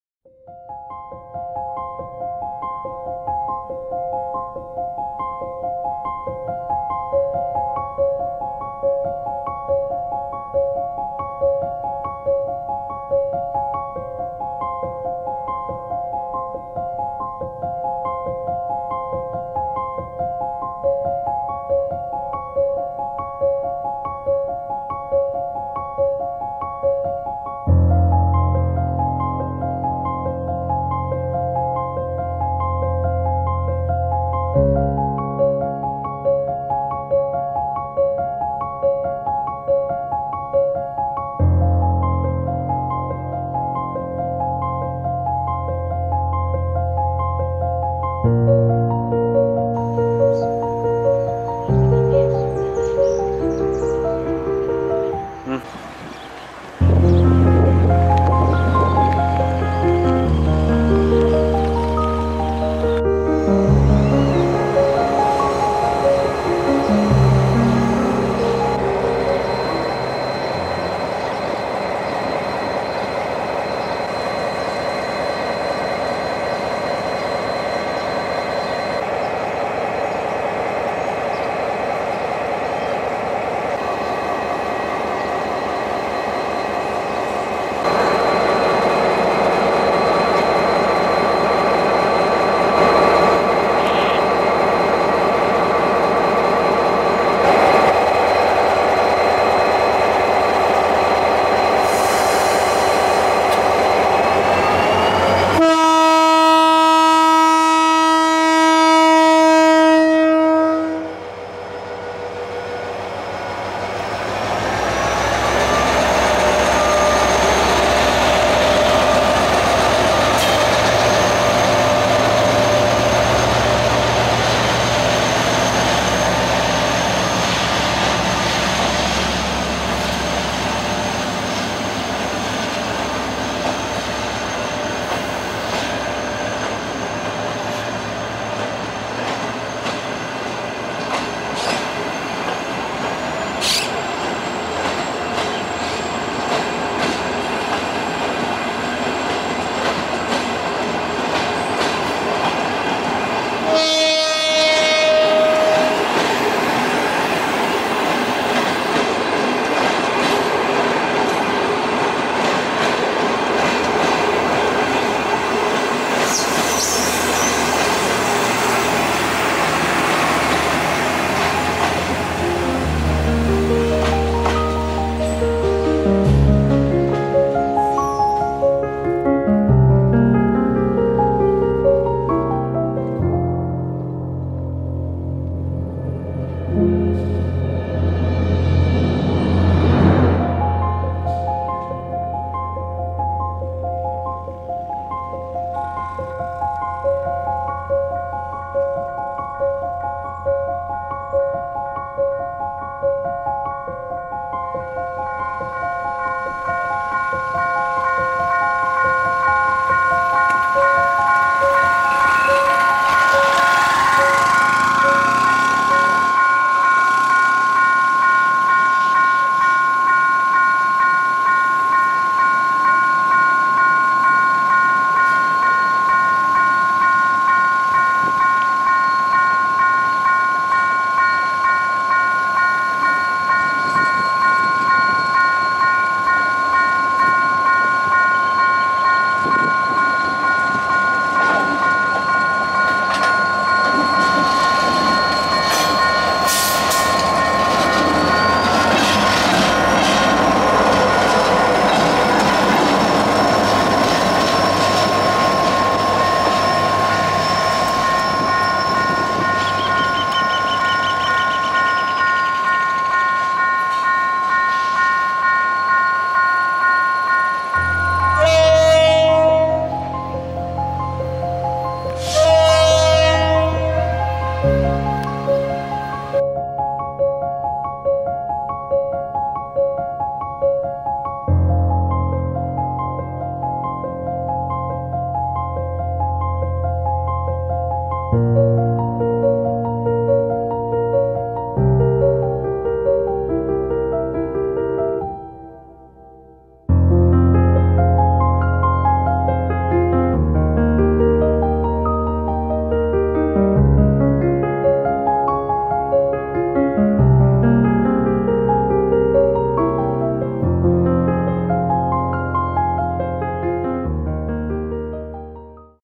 ¡¡OTRO DÍA ACCIDENTADO!! ¡¡BARRERAS URGENTES!! Accidente fatal en BRANDSEN 22.12.2023
¡Buenas buenas para todos! Espero que se encuentren muy bien. Durante la semana tuvo lugar un nuevo accidente en el paso a nivel de la vía Mar del Plata del kilometro 68 de la vía descendente. La A915 que comandaba el tren 804 procedente de Chascomús con destino Korn, impacto contra un vehículo Ford Taunus. El auto era conducido por una persona femenina de 43 años que falleció en el acto.

Volvemos a RECALCAR y pedir nuevamente barreras para este paso a nivel. Es de suma importancia concientizar, pero también son mas que importantes las medidas de seguridad fundamentales, como por ejemplo, barreras, chicharras, etc. NO PUEDEN SEGUIR EXISTIENDO MAS ACCIDENTES EN ESE LUGAR.